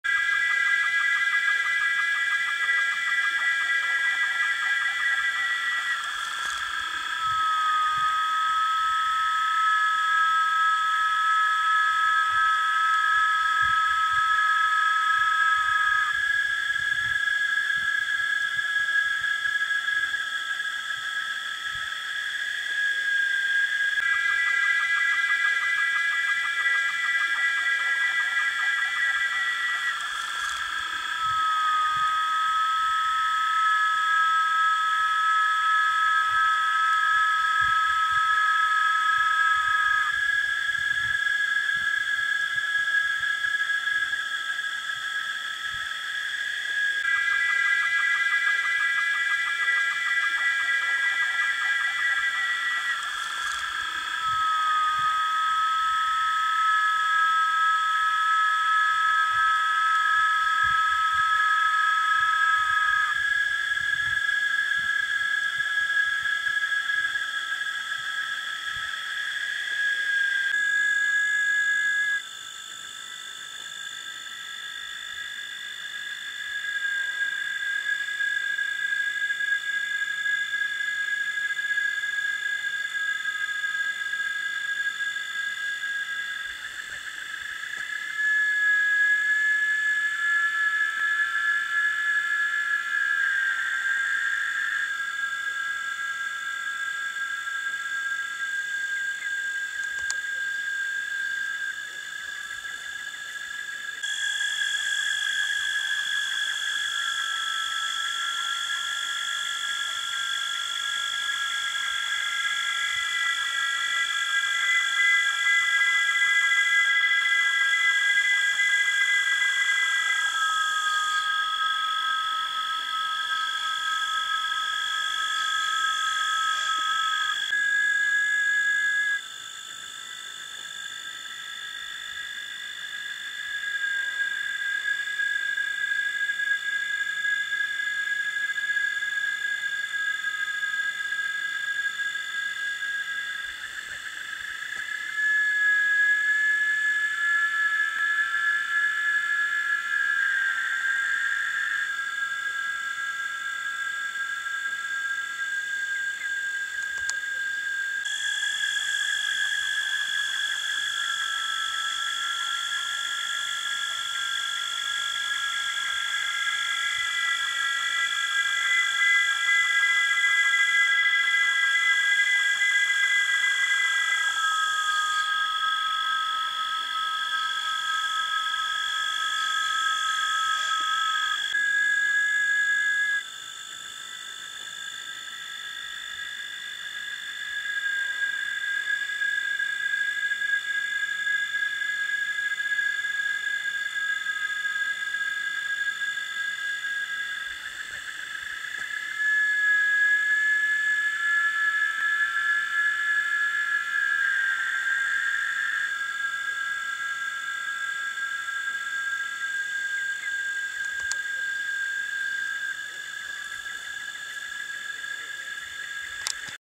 QUEEN VIRGIN IN MATING FLIGHTS, ANT, ANTHILL (ATTA), FORMIGA-SAÚVA, Ant works, Cicada sings.
Anthill. Scientific name: Atta. Brazilian name Formiga saúva. Kingdom: Animalia, Phylum: Arthropoda, Class: Insect, Order: Hymenoptera, Family: Formicidae. Feeding: These leaf-cutting ants cut the leaves into pieces, carry them inside their nests, storing them in humid chambers, serving as a substrate for the development of a mutualistic fungus, which constitutes their exclusive food. The exact identity of this fungus varies according to the species involved, with the following species listed: Leucoagaricus gongylophorus, Attamyces bromatificus, Pholota gongylophora (Moeller). Geographical distribution: Native to the New World and most abundant in the Neotropics.